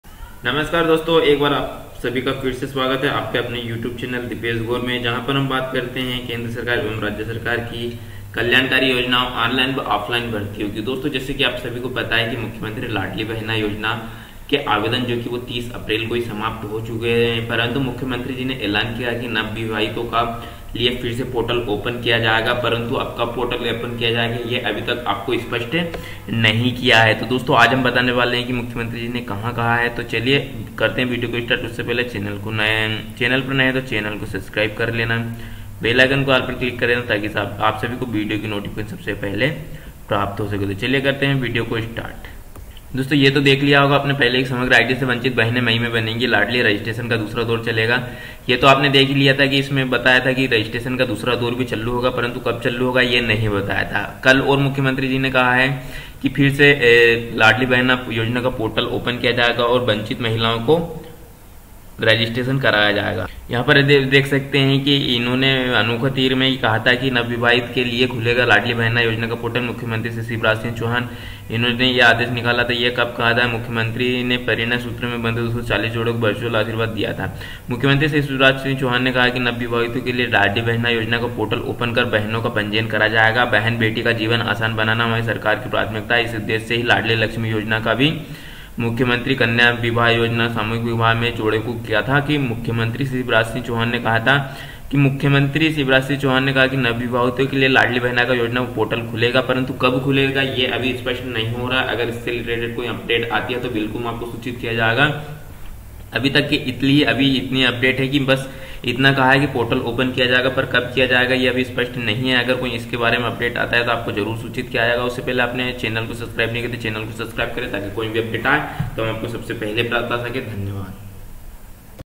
फिर से मई में बनेगी बहने बनेगी लाडली पात्र हितग्राही महिला योजना से लाभान्वित करेगे
लाडली बहना योजना आदेश जारी पात्र व नव विवाहितो के लिए फिर से शुरू होगा पोर्टल @dipeshgour

My others social media link follow

      😍DIPESH GOUR😍